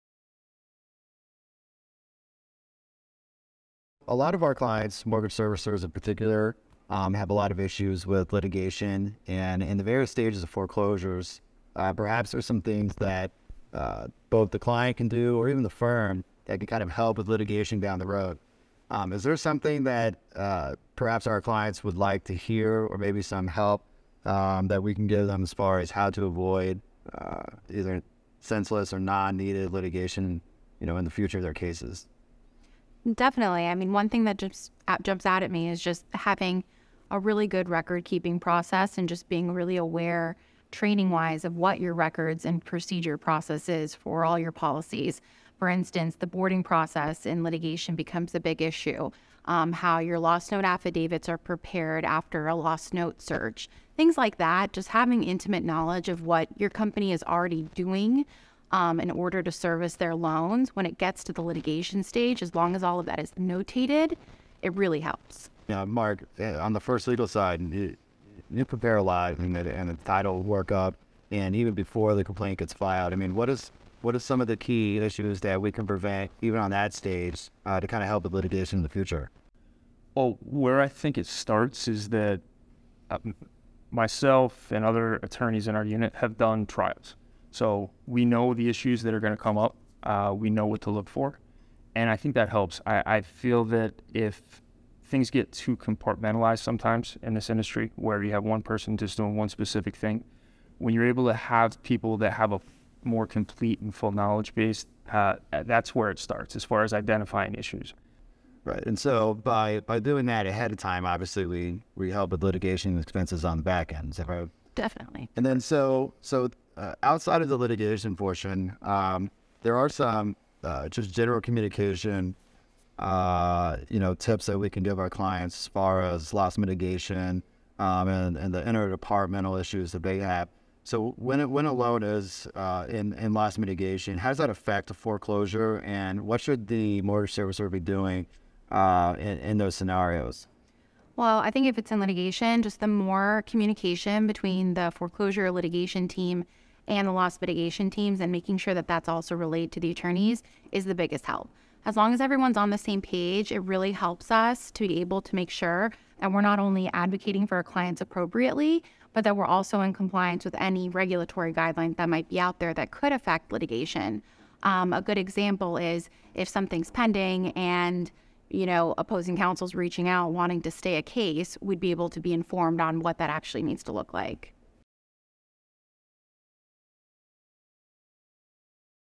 How to Avoid Litigation and Mitigate Risk
Side Bar with Kelley Kronenberg
Mortgage Banking, Foreclosure, and Lending Services: How to Avoid Litigation and Mitigate Risk
Jason M. Vanslette, Marc A. Marra, and Jacqueline Costoya Guberman